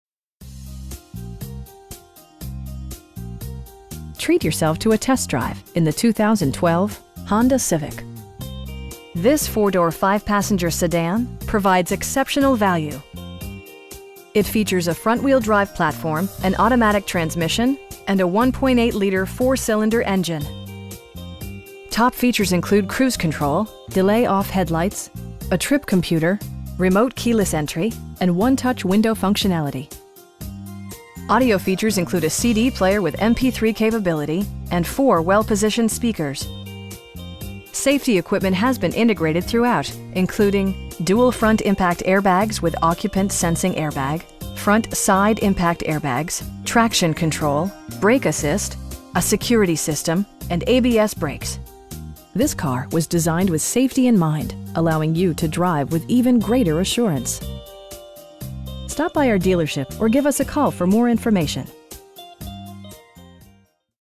2012 Honda Civic LX in Rock Hill, SC 29730
Honda Cars of Rock Hill
686 Galleria Boulevard

Treat yourself to a test drive in the 2012 Honda Civic This 4 door, 5 passenger sedan provides exceptional value! It features a front-wheel-drive platform, an automatic transmission, and a 1.8 liter 4 cylinder engine. Top features include cruise control, delay-off headlights, a trip computer, front bucket seats, air conditioning, tilt steering wheel, remote keyless entry, and 1-touch window functionality. Audio features include a CD player with MP3 capability, and 4 speakers, providing excellent sound throughout the cabin. Safety equipment has been integrated throughout, including: dual front impact airbags with occupant sensing airbag, front side impact airbags, traction control, brake assist, a security system, and ABS brakes. This car was designed with safety in mind, allowing you to drive with even greater assurance. We pride ourselves in consistently exceeding our customer's expectations. Stop by our dealership or give us a call for more information.